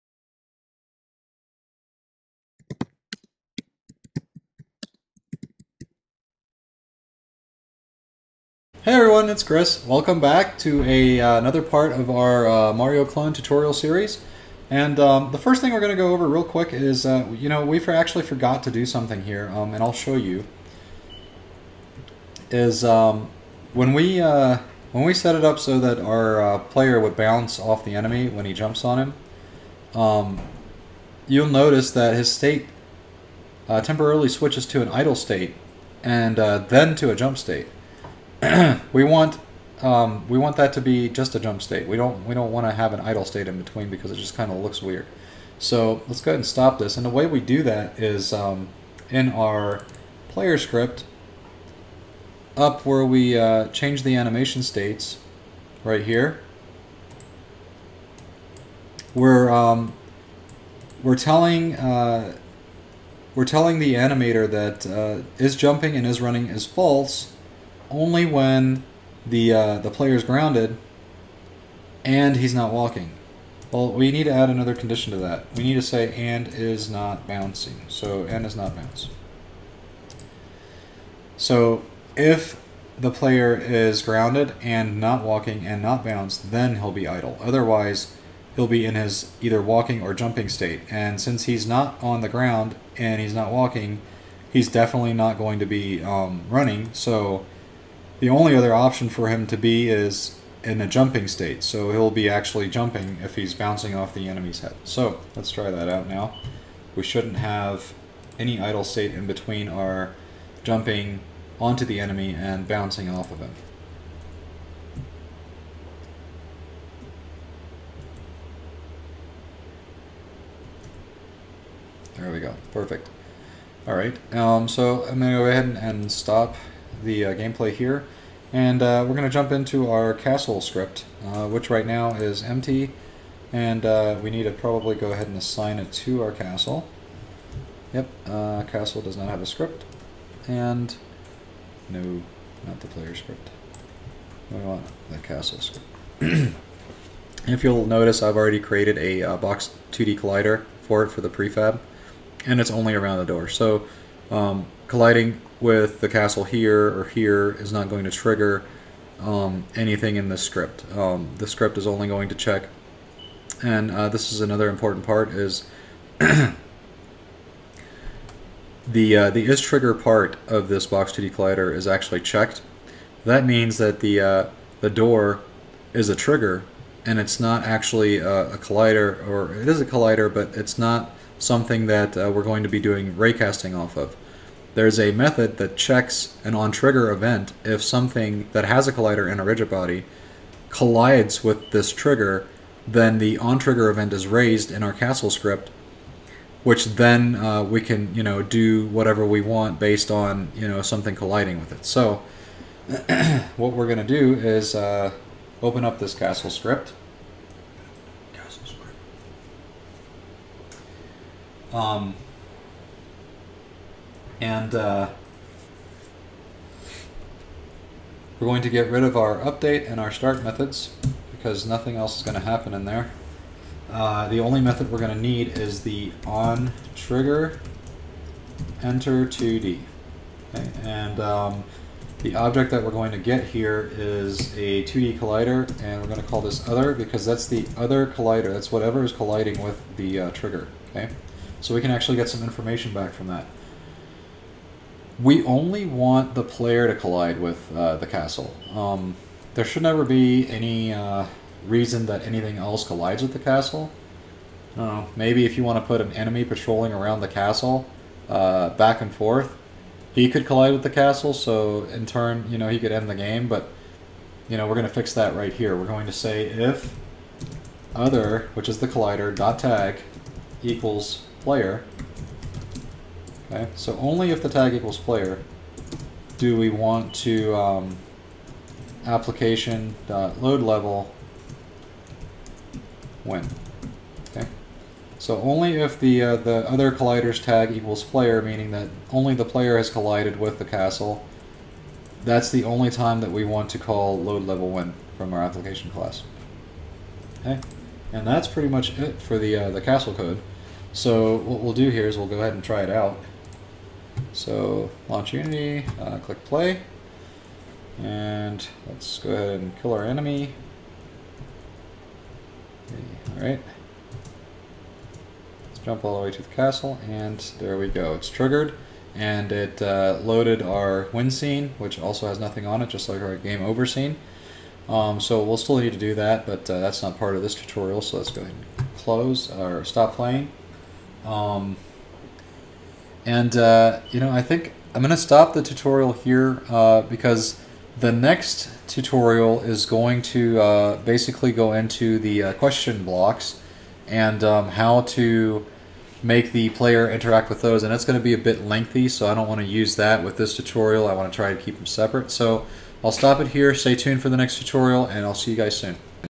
How to make a platformer like Mario in Unity - Part 10 - Castle & Winning
In part 10 of our series we cover how to create a collision detection system that will detect when the player collides with the castle. We then handle loading another scene to indicate that the player has won.

► Your donations help pay my bills and allow me to make great content.

► You might also like to watch "How to make a game like Pac-Man in Unity - Part 1"